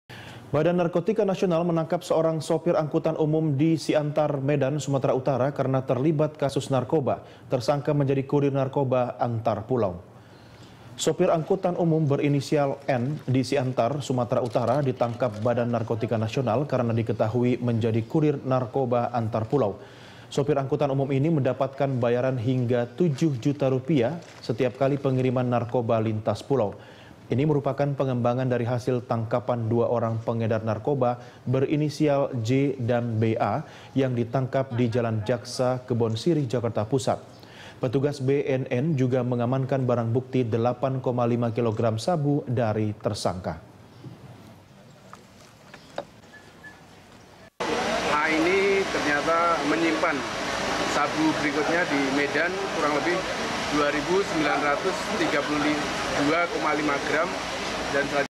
BNN Menangkap Kurir Narkoba Antarpulau
Badan Narkotika Nasional (BNN) meringkus seorang sopir angkutan umum di Siantar, Medan, Sumatera Utara, karena terlibat kasus narkotika dan obat-obatan berbahaya (narkoba). Tersangka menjadi kurir narkoba antarpulau.

Facebook.com/BeritaSatuTV
Youtube.com/BeritaSatu